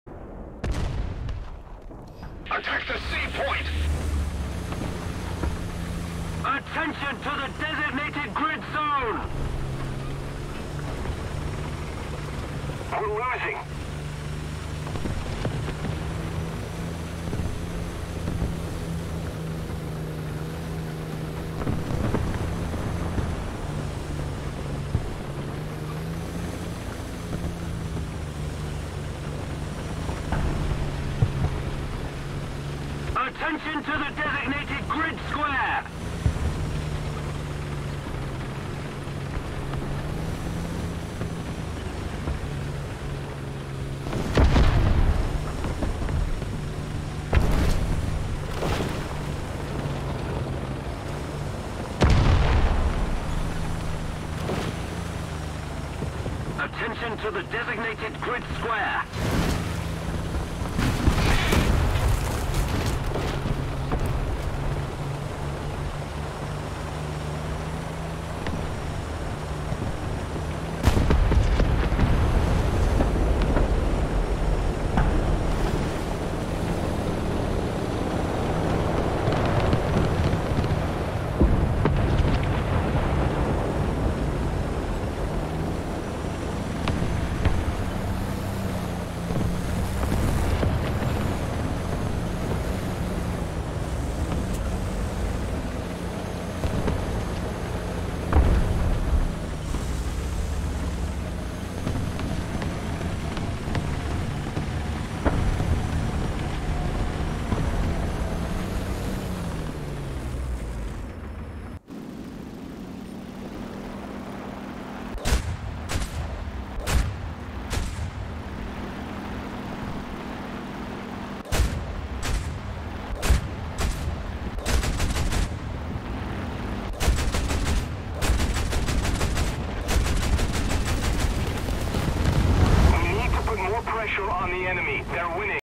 lead up to african___santa getting caught hacking again
this was my pathing to the spot i hid in to activate my plane (which in the next video you can see the hacker african___santa) not only pin pointing my location but climbing a hill that is too steep for any vehicle to get up to get to me